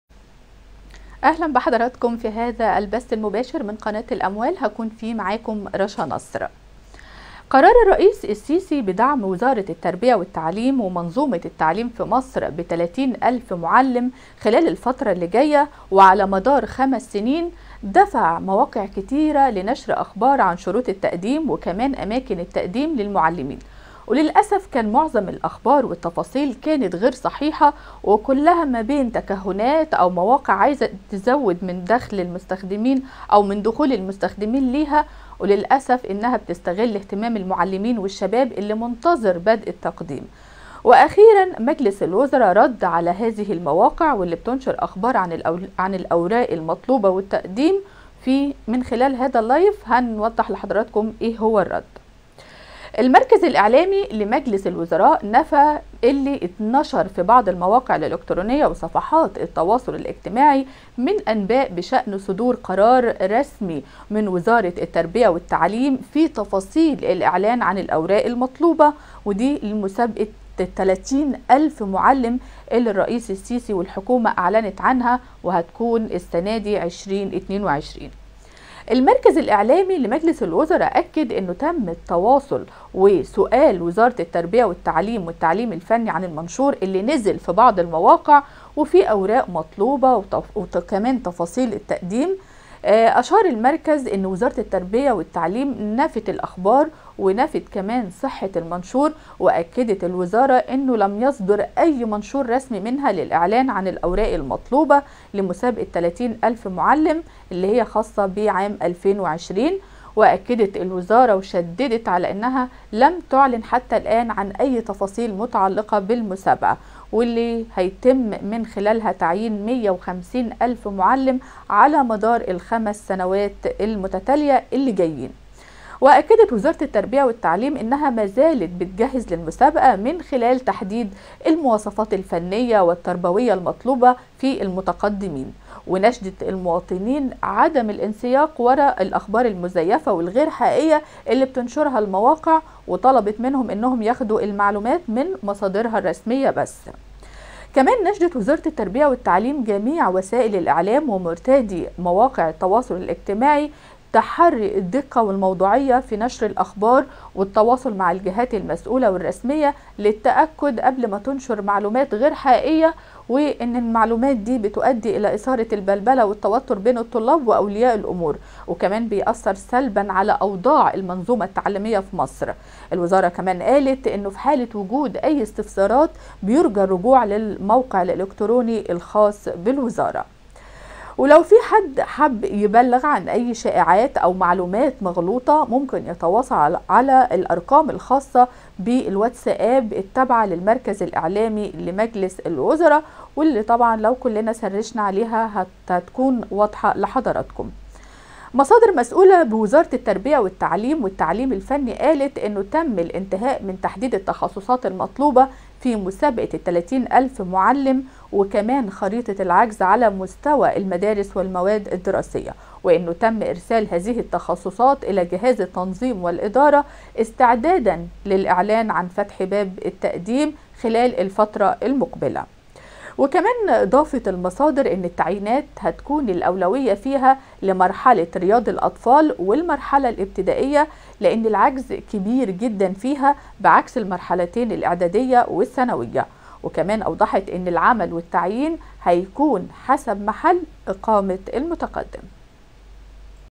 الحكومة تكشف تفاصيل جديدة عن مسابقة 30 ألف معلم ومفاجأة بشأن صدور منشور للإعلان عن الأوراق المطلوبة!
قرار الرئيس السيسي بدعم وزارة التربية والتعليم ومنظومة التعليم في مصر ب30 الف معلم خلال الفترة اللي جاية ، وعلى مدار 5 سنين ، دفع مواقع كتيرة لنشر اخبار عن شروط التقديم واماكن التقديم للمعلمين، وللاسف كان معظم الاخبار والتفاصيل كانت غير صحيحة وكلها ما بين تكهنات او مواقع عايزة تزيد من دخول المستخدمين لها وللاسف انها بتستغل اهتمام المعلمين والشباب اللي منتظر بدء التقديم ، واخيرا مجلس الوزراء رد على هذه المواقع واللي بتنشر اخبار عن الاوراق المطلوبة والتقديم ..